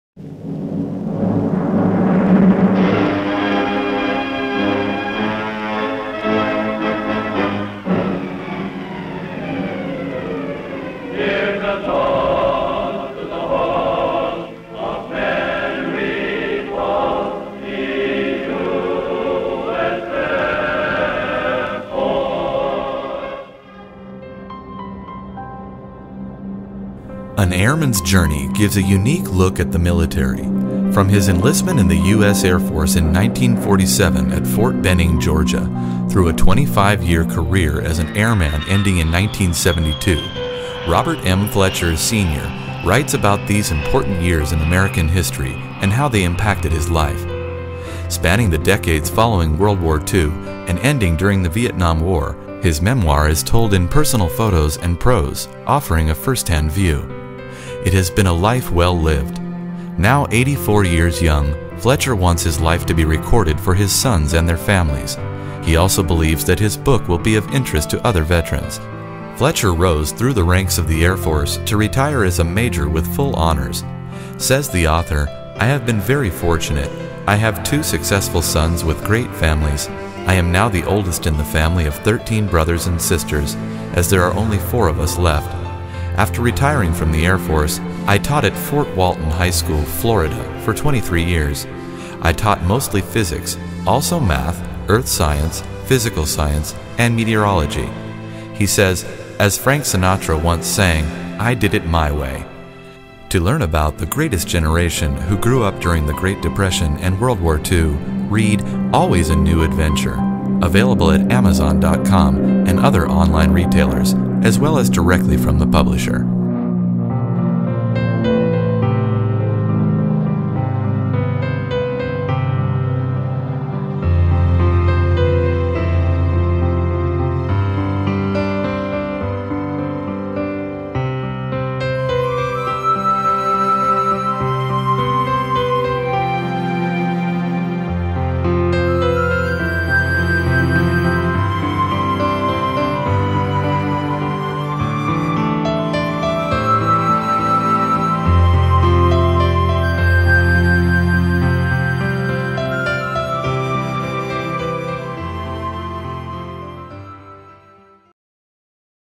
An Airman's Journey by Robert M. Fletcher
Hard Cover
8.50 x 8.50 inches
Premium Color/White
222 pages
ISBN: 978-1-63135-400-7
Price: $49.00

Title: An Airman's Journey
Subtitle: From 1947 Enlistment Through 1972 Retirement Always a New Adventure
Author: Robert M. Fletcher, Sr.

From enlistment at Fort Benning, Georgia in 1947 to Las Vegas, Warner Robins, post-war Japan, to Korea, Germany, and Okinawa, Robert Fletcher rose through the ranks of the US Air Force and proudly retired as a Major with full honors.  His chosen field was meteorology and he later became a high school physics teacher.

 Truly “A life well lived”.